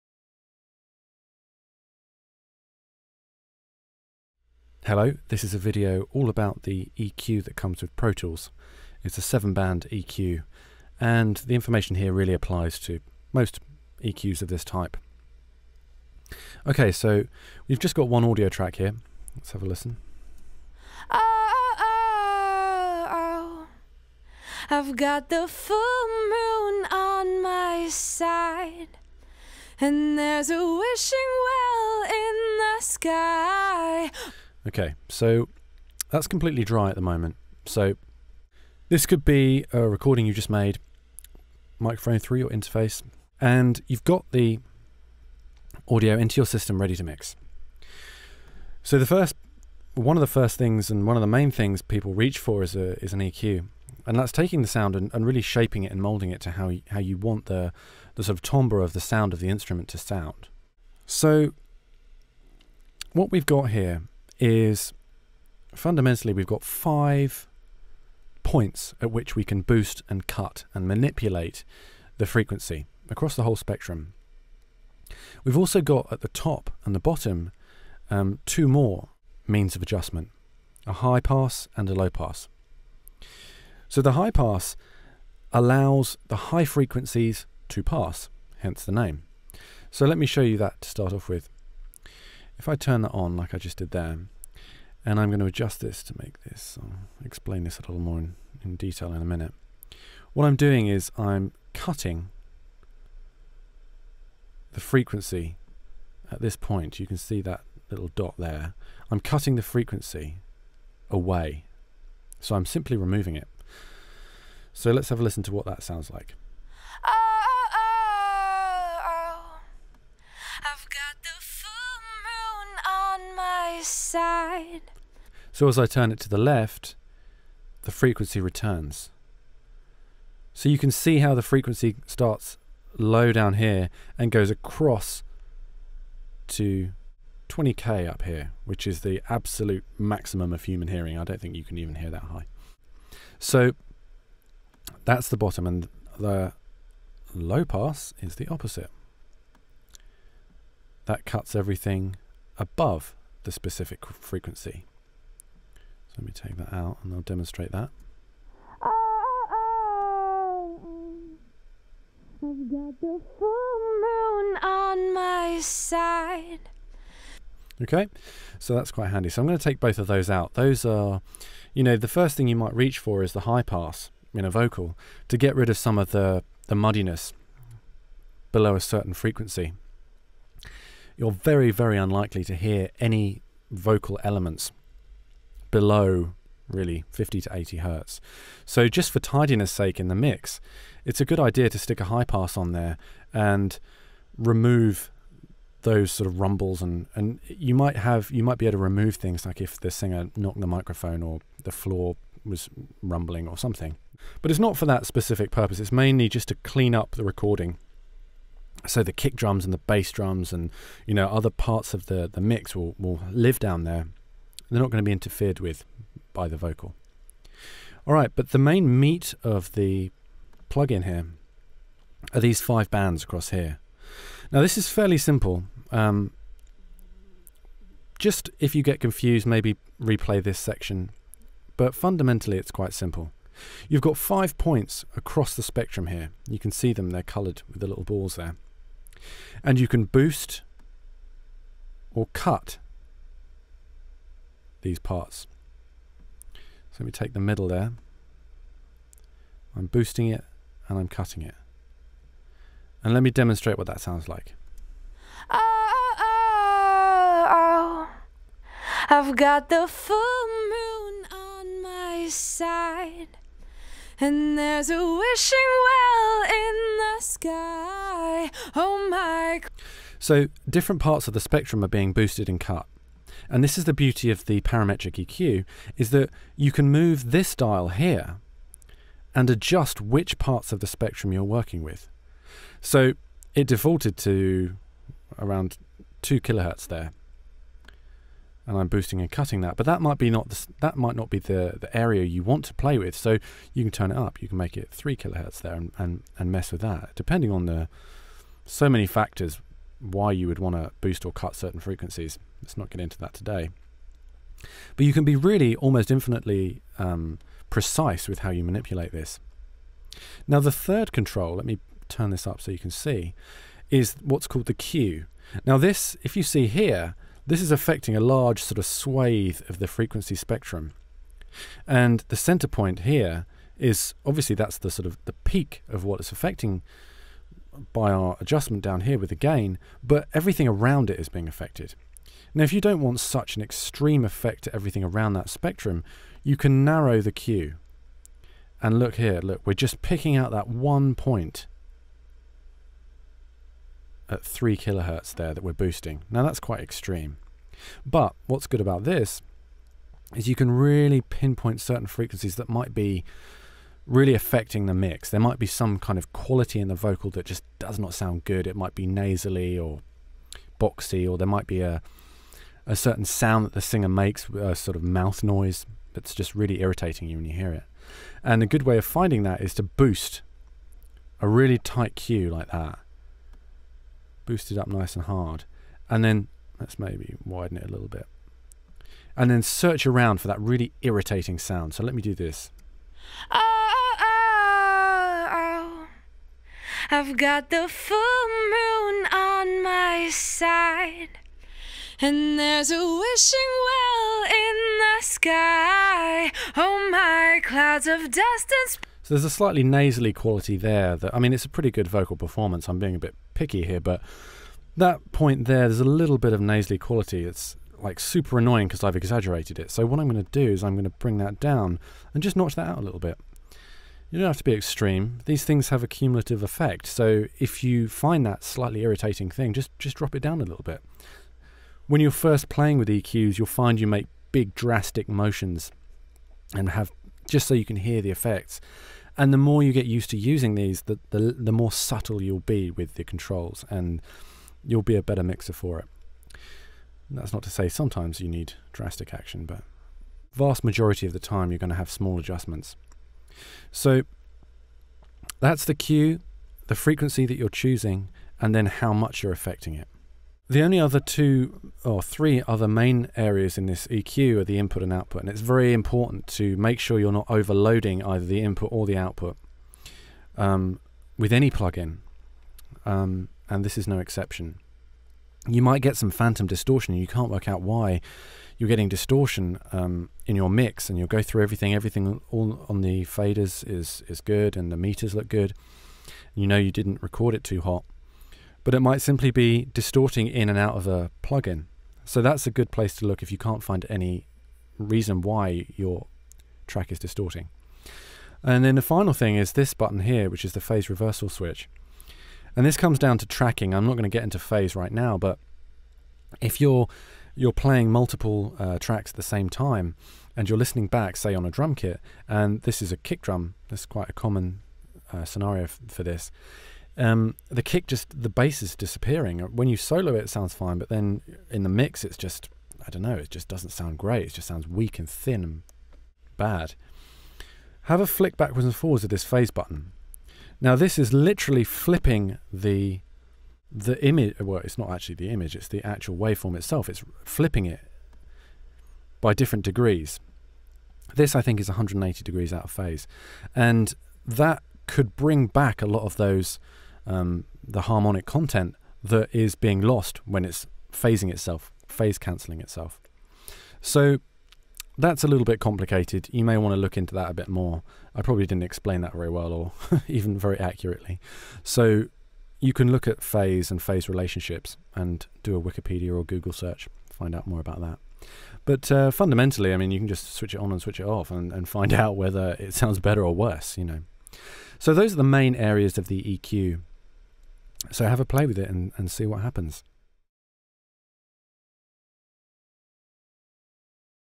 EQ in Pro Tools
A video explaining how the seven band parametric equalizer in Pro Tools works.